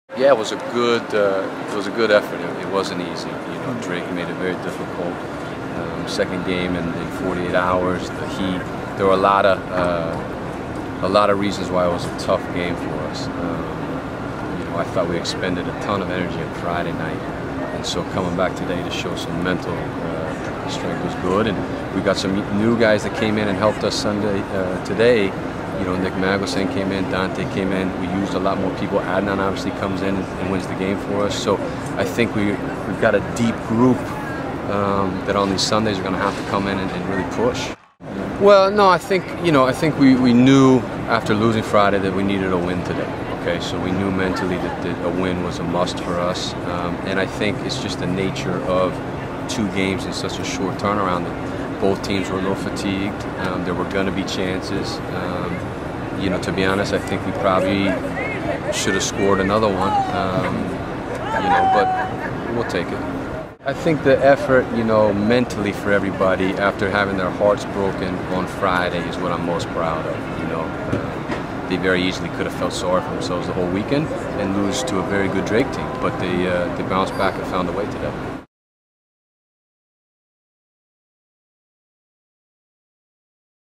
Men's Soccer: Billikens vs. Drake recap
Recap of the SLU men's soccer match against Drake. The Billikens won 1-0 on a goal by Adnan Gabeljic.